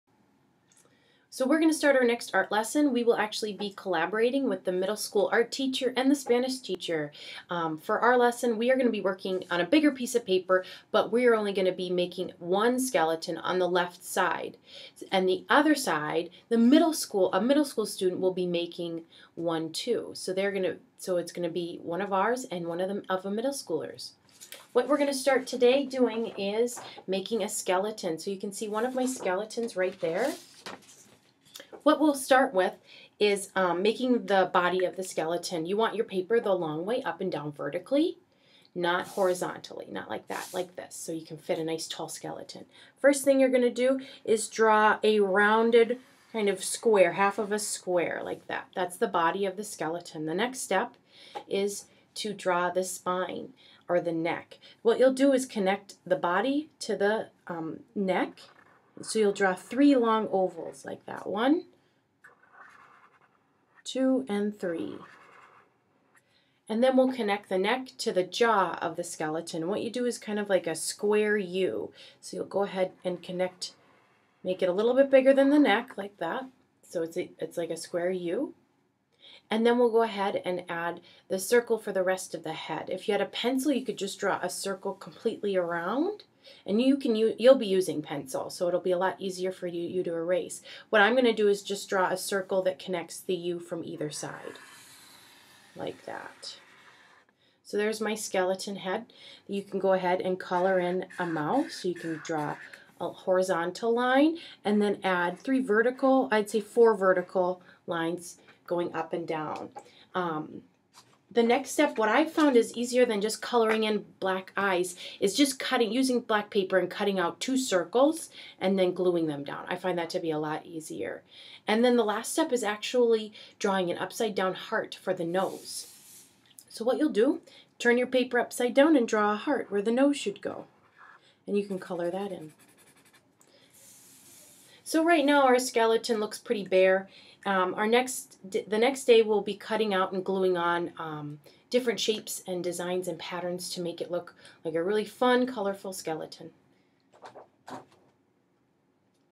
Drawing Skeletons: Day of the Dead
4th Grade Art Lesson for Dia de los Muertos